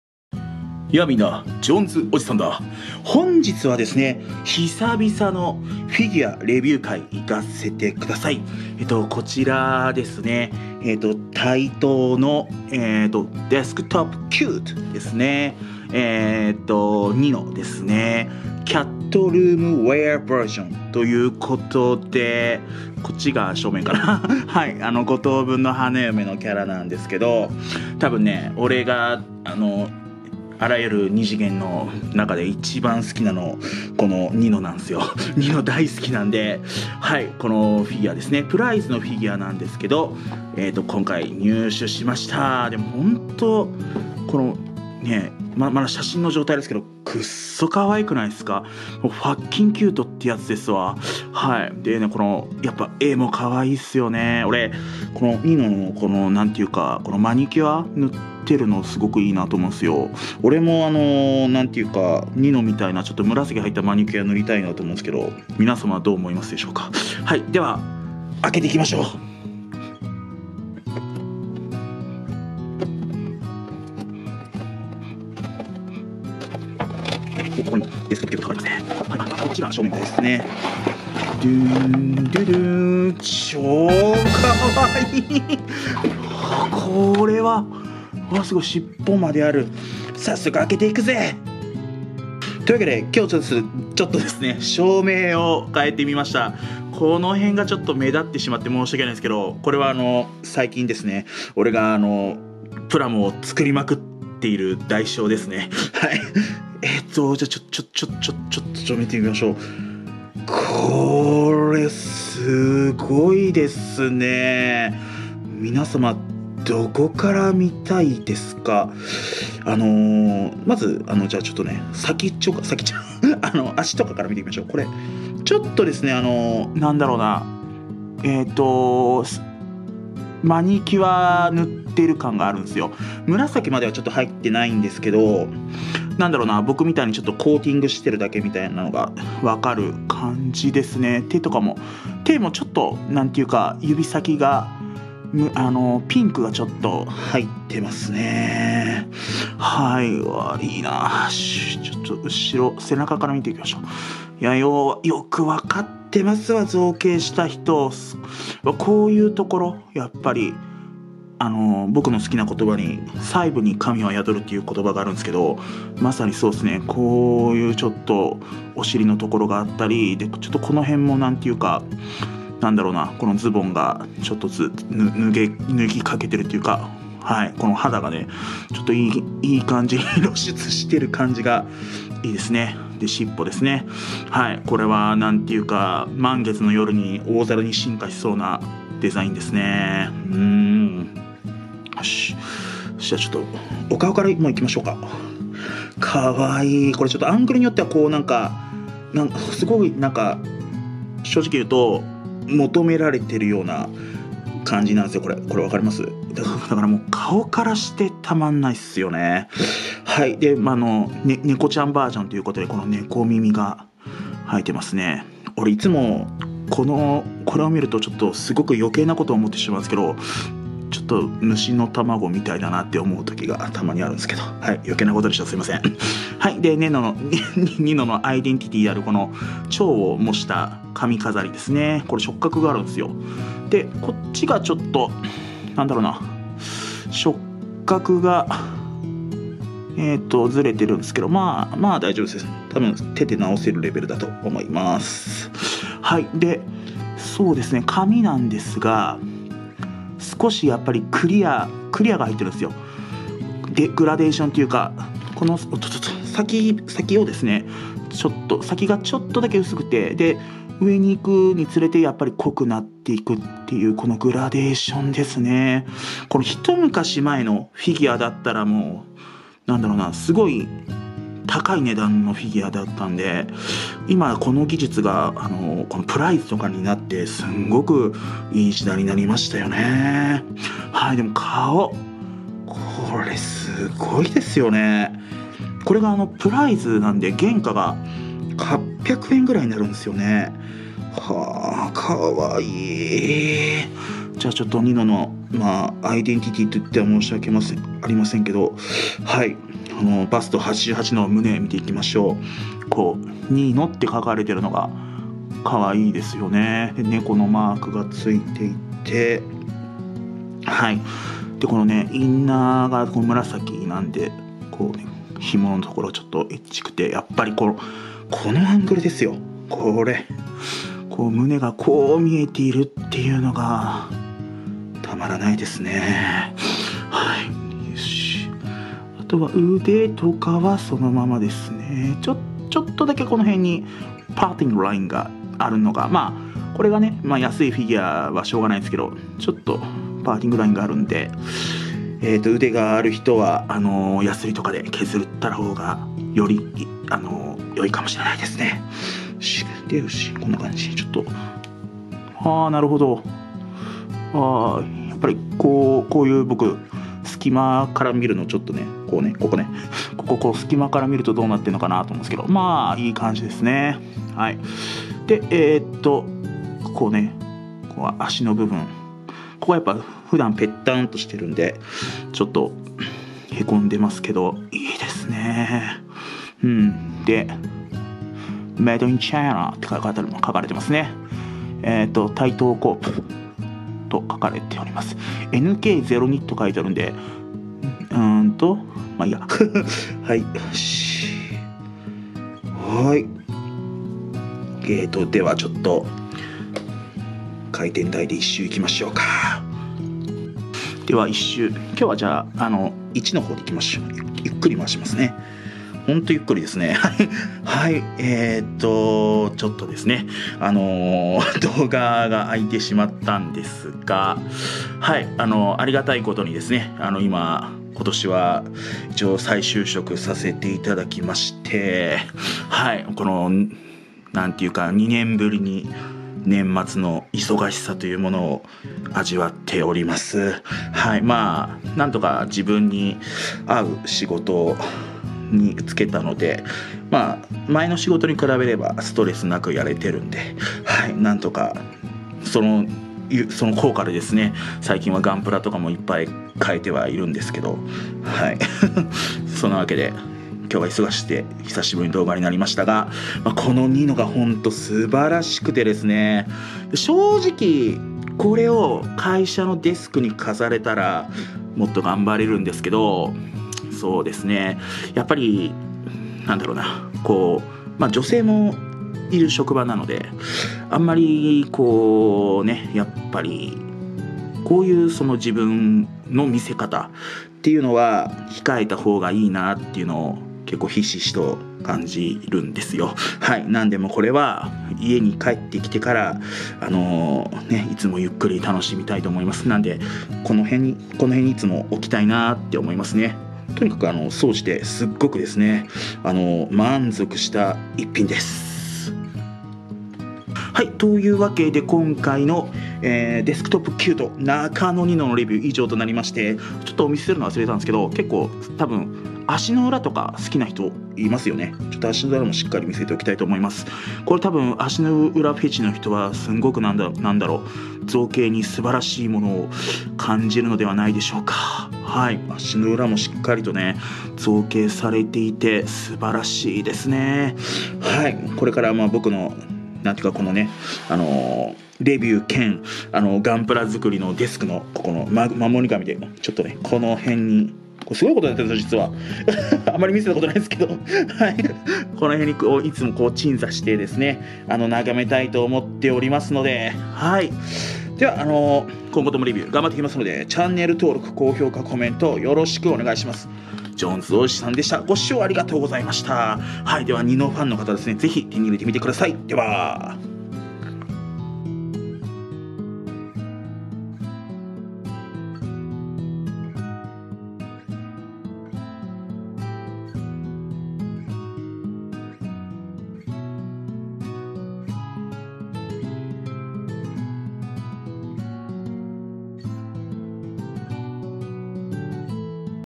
【五等分の花嫁】中野 二乃 描き下ろしcat room wear ver 【TAITO】desk top cute figure プライズフィギュア 竹達彩奈 春場ねぎ
二乃の可愛いフィギュアがプライズで登場したとの情報を聞き、早速GET！
あまりの可愛さに悶絶です！
このフィギュアのお陰で年末の疲れも吹っ飛びました！！

本動画は14歳以上の大人の方を対象としています。

案件、デジラマ等製作依頼はこちらへ